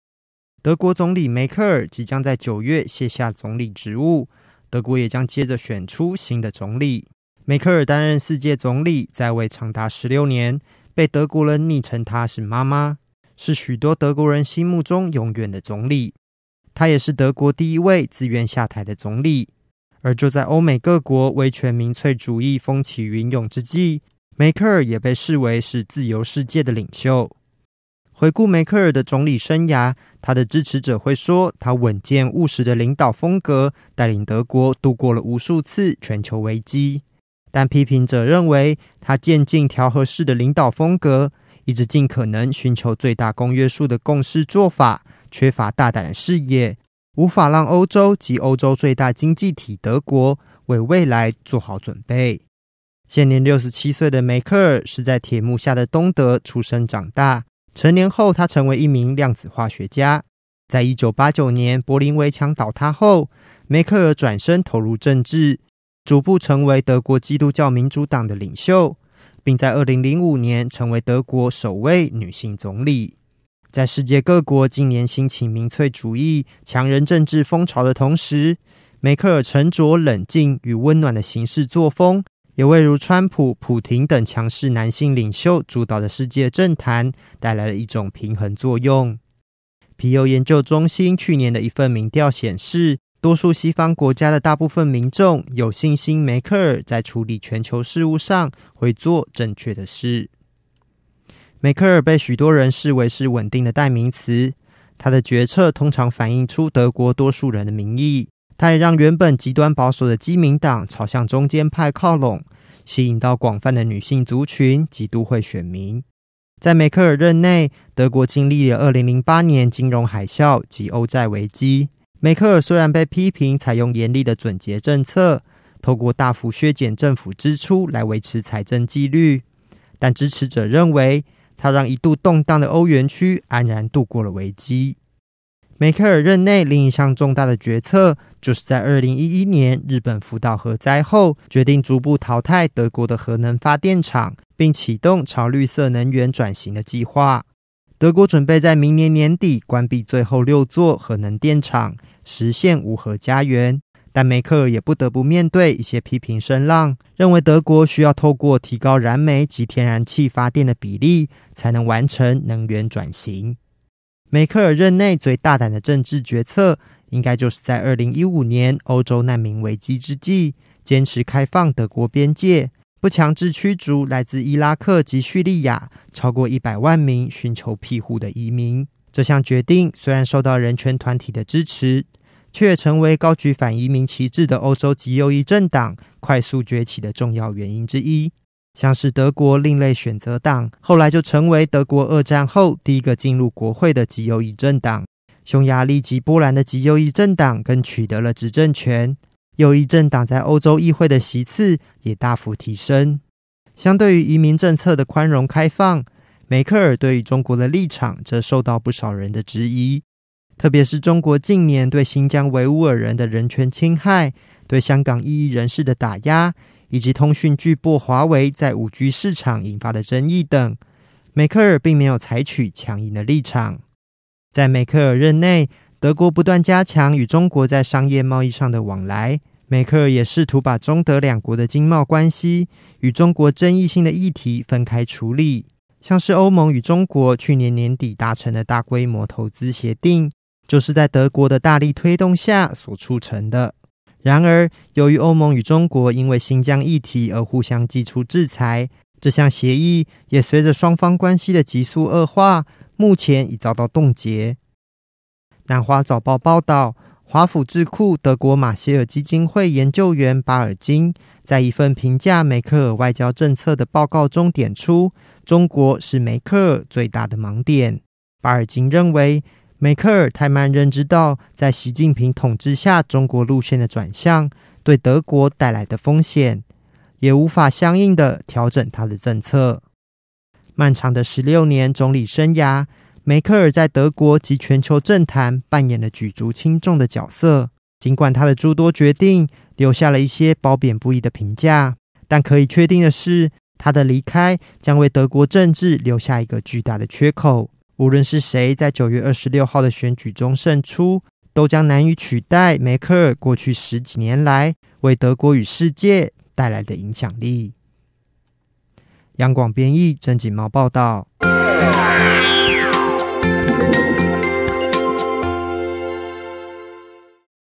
16年總理生涯將落幕 德國媽媽梅克爾務實作風樹立典範【央廣國際新聞】
德國總理梅克爾(Angela Merkel)即將在9月底正式卸任，結束她長達16年的總理生涯。梅克爾冷靜的行事作風，在多場關鍵危機中，帶領德國及全世界度過了難關，也讓她成為過去數十年來全球最具影響力的領袖之一。